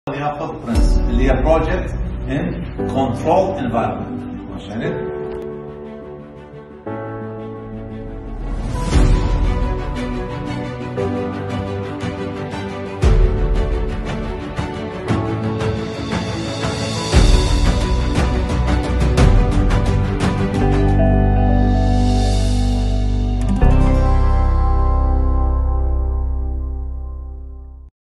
دورة أسس ادارة المشاريع الادراية
نظمت مؤسسة الفجيرة للموارد الطبيعية متمثلة بادارة الموارد البشرية برنامجاً تدريبياً حول أسس إدارة المشاريع  الإدارية ، و يستمرالبرنامج لمدة يومين متتاليين .